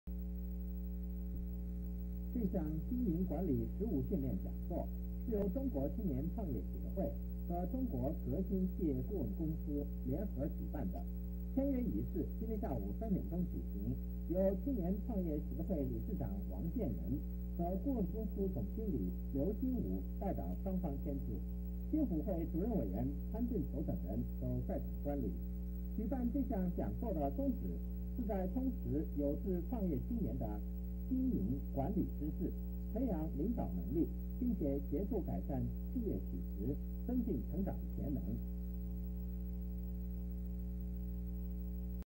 經理管理實務訓練講座簽約儀式
(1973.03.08)經理管理實務訓練講座簽約儀式.由中國青年創業協會和中國革新企業顧問公司聯合主辦